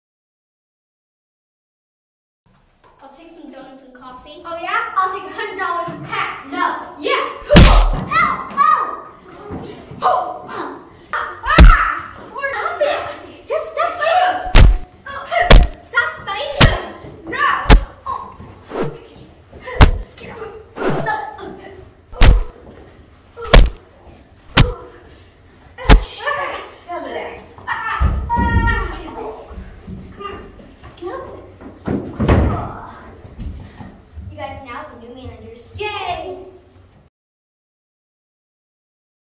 The little cafe
From our Nov Street week. The students had to choreograph their own fight scene.